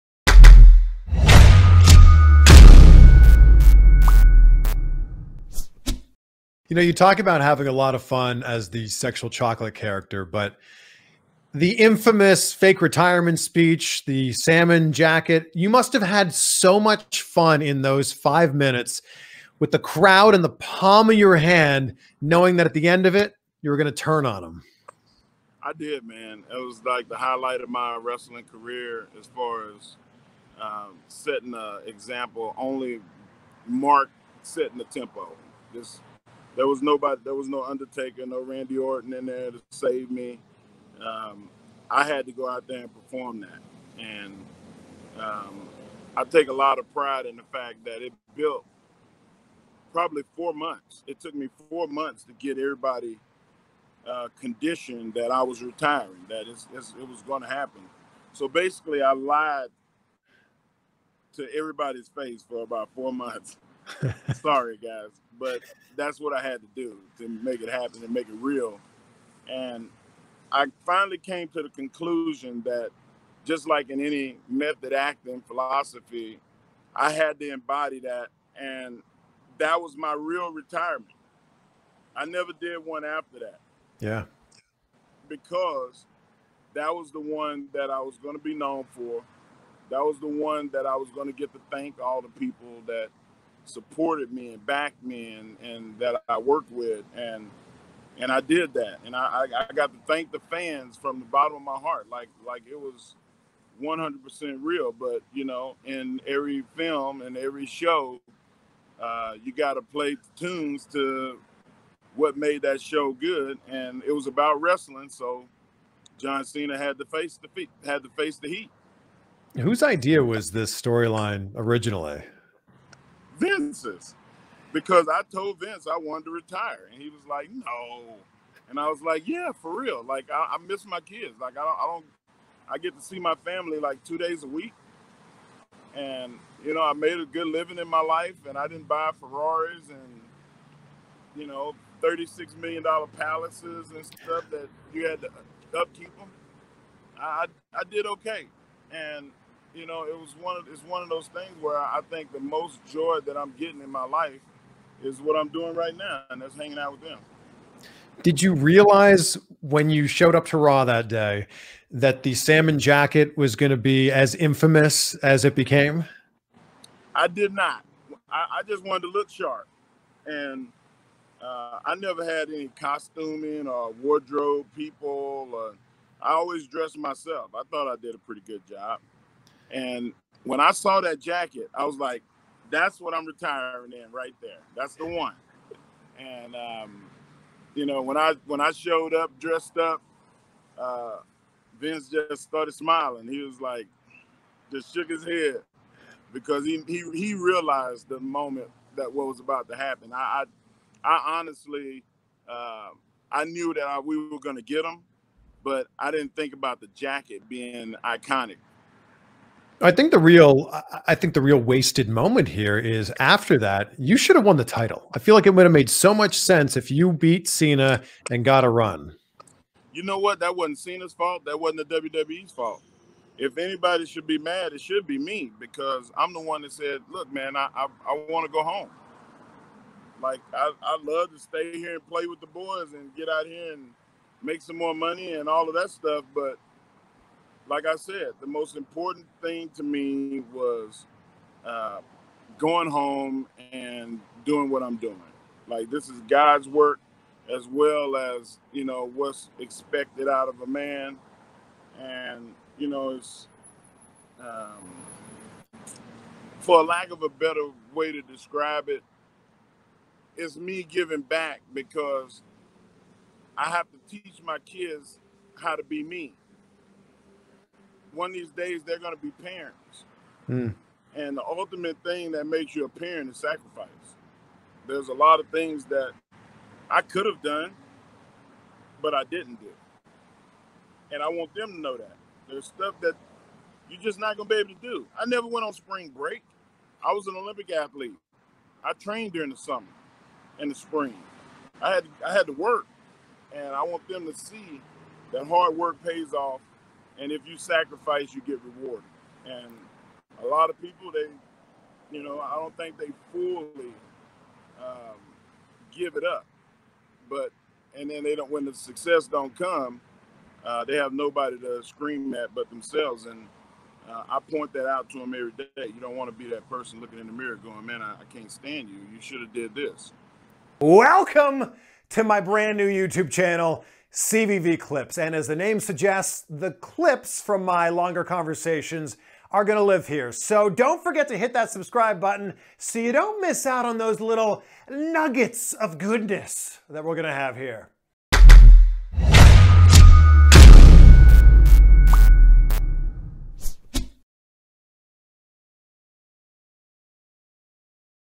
Mark Henry on his fake retirement speech and the salmon colored jacket!
Mark Henry goes into detail about his fake retirement speech and how he chose the salmon colored jacket that he wore that night.